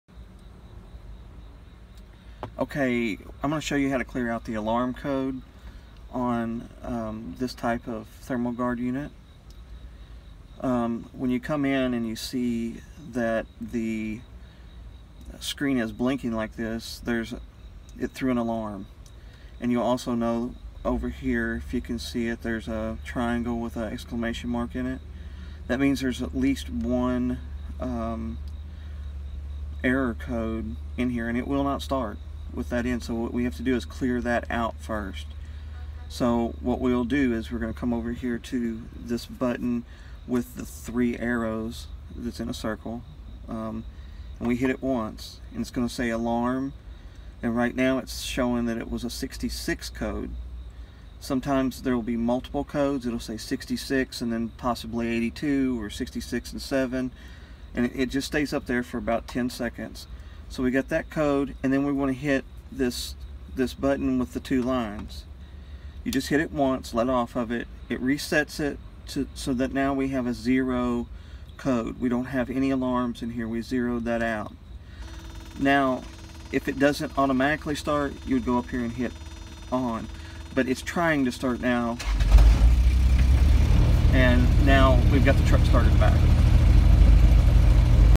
Reset alarm on refrigerated box trucks
How to clear the alarm codes on the thermo king refrigerated units in order to restart the refer.